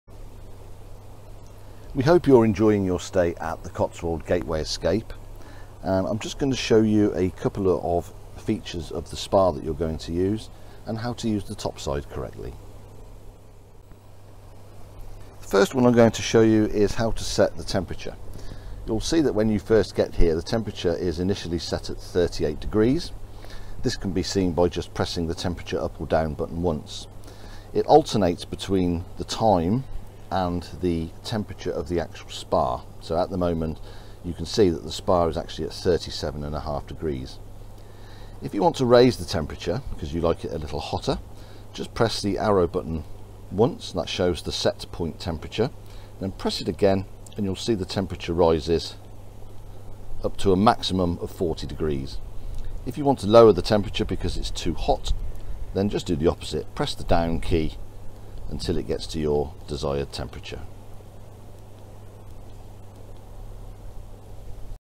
Cotswold Gateway Escape Setting the Temperature
Details on setting the Temperature on your Cotswold Gateway Spa.